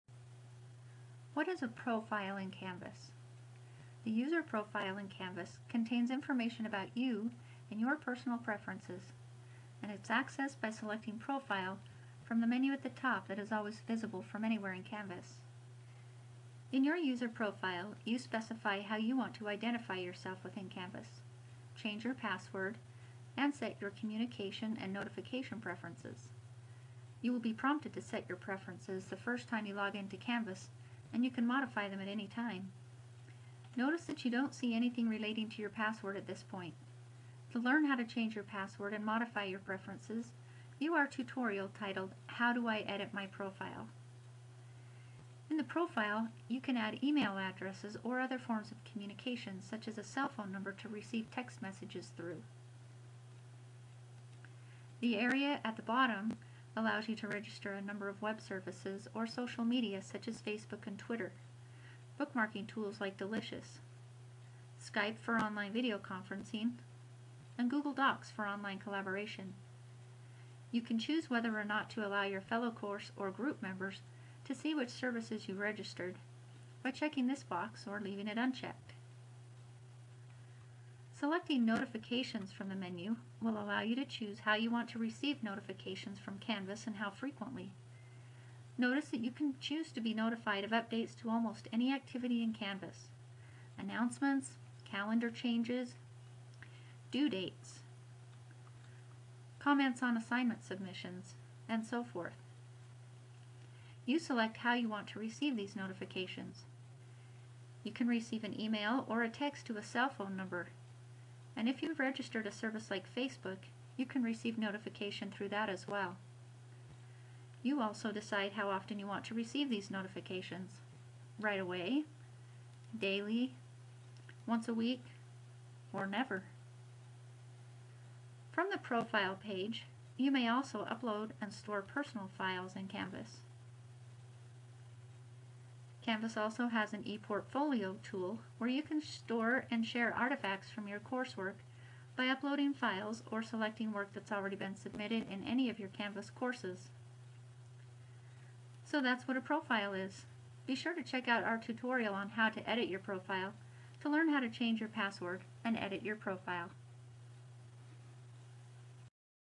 What is a profile?
This tutorial describes the user profile in Canvas.